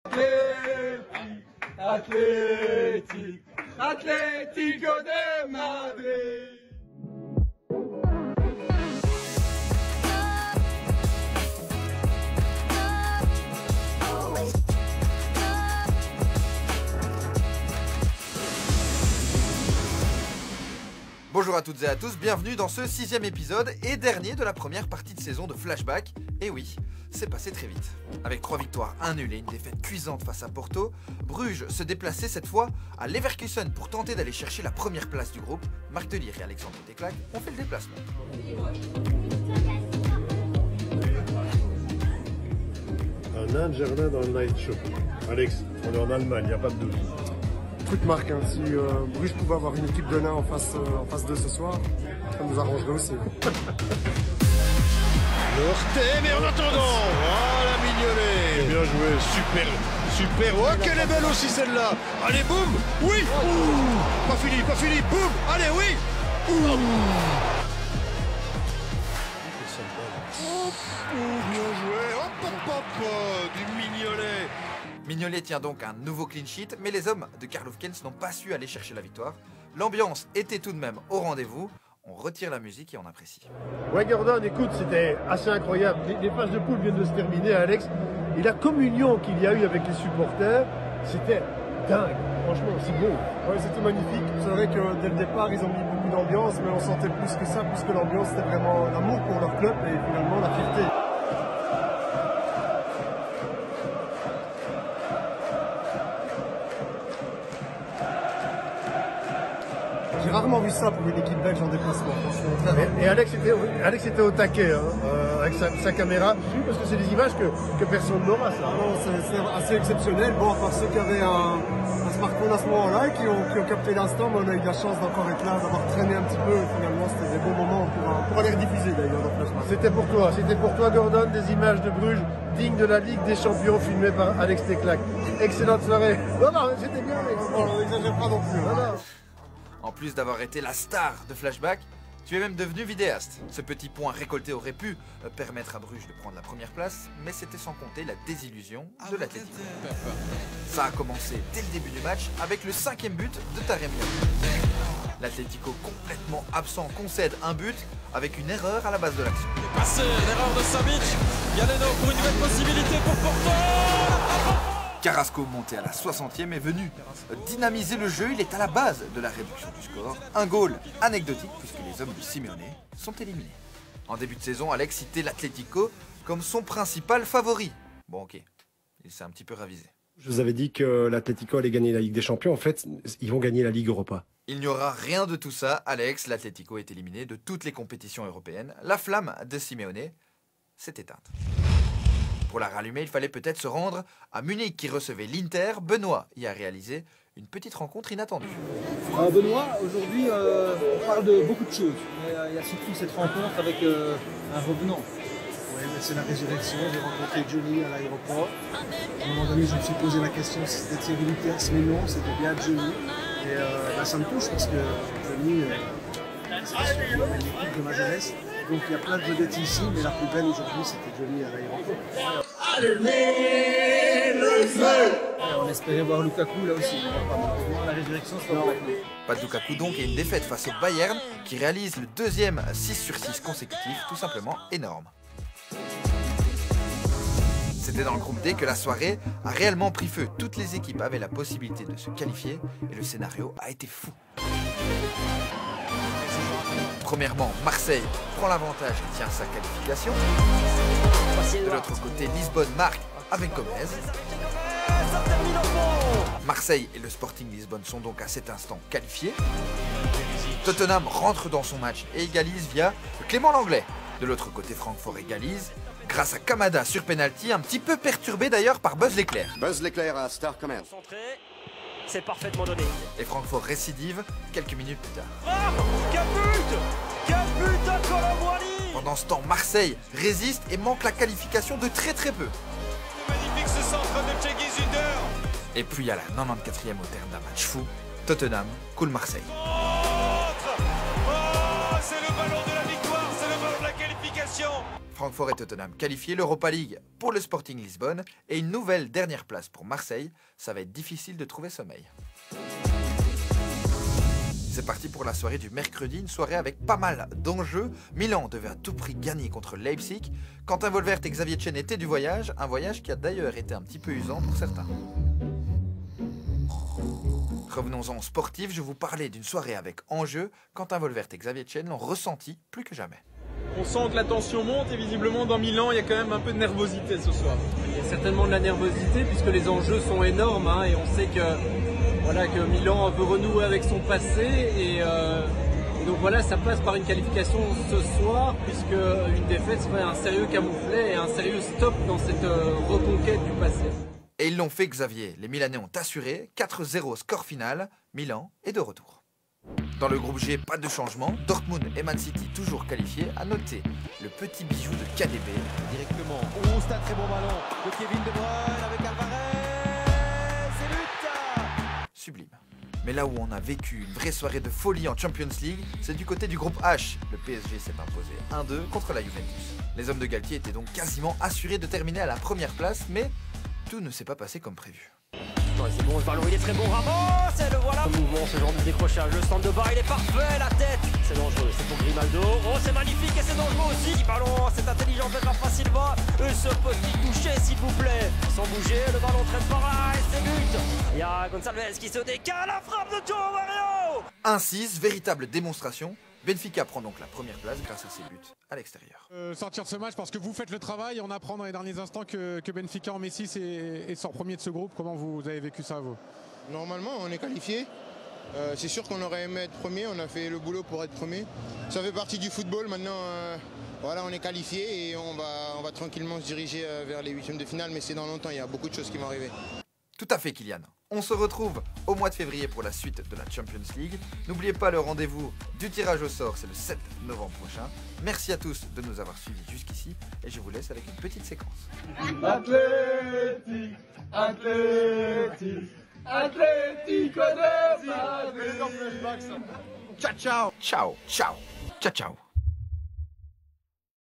𝗙𝗹𝗮𝘀𝗵𝗯𝗮𝗰𝗸 – 𝗘𝗽𝗶𝘀𝗼𝗱𝗲 𝟲 ✈️
La phase de groupes la plus intense de l’histoire de la Ligue des champions est terminée !
✅ Marc Delire et Alexandre Teklak ont été impressionnés par l’ambiance accordée aux Blauw & Zwart ! De quoi consoler Alex qui a définitivement eu 0 sur toute la ligne concernant ses propos d’avant-saison.
✅ Bernard Bolly et Benoit Thans ont eu la chance de rencontrer un revenant lors de leur voyage vers Munich où Lukaku était absent.
✅ Et enfin, Quentin Volvert et Xavier Chen ont assisté à la qualification de Milan !